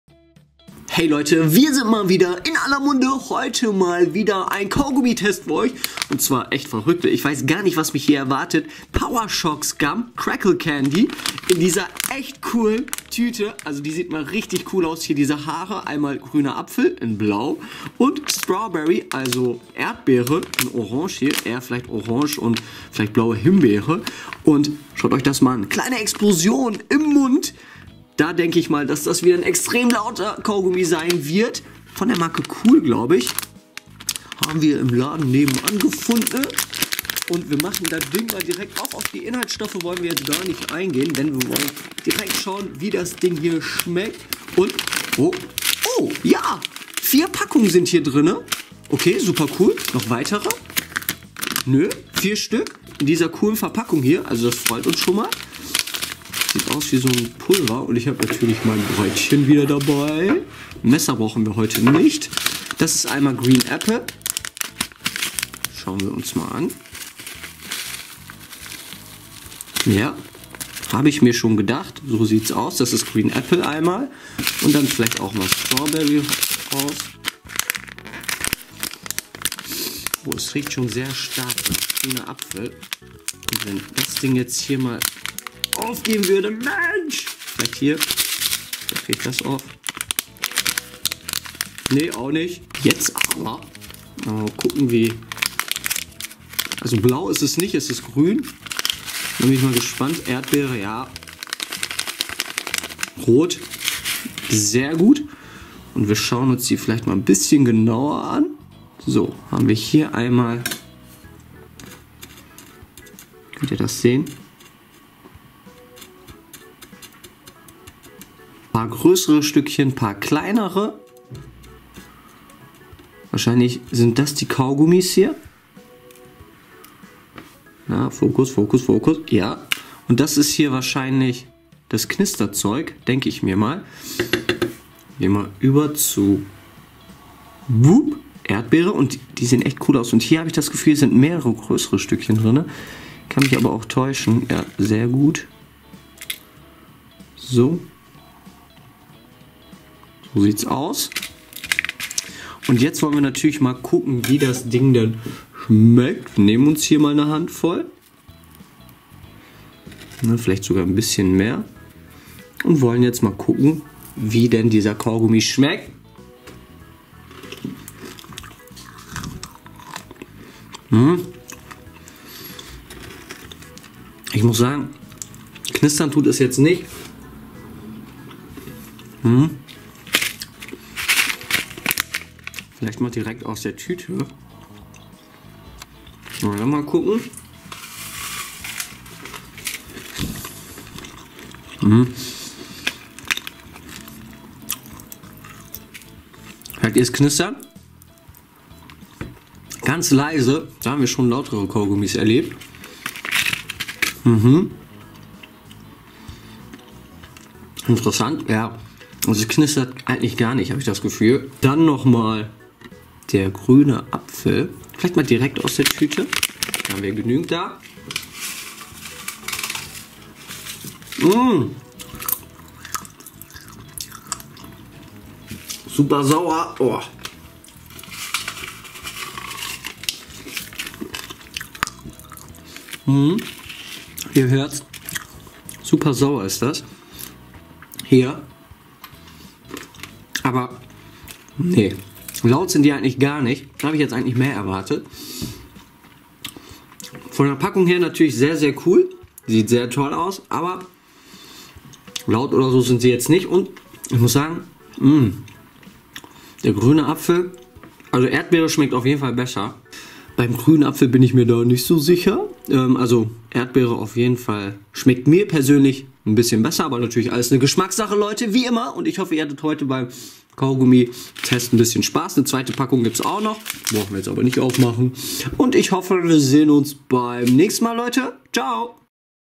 POWER SHOCKS KAUGUMMI Süßigkeiten Test - Ich bin nicht schockiert! Candy Test
Jeden Tag gibt es was Neues zu futtern! Süßigkeiten, Burger, Energy Drinks und alles was das JUNKFOOD Herz begehrt!

Wir probieren ALLES was man essen kann (aber nicht unbedingt essen sollte)!

4028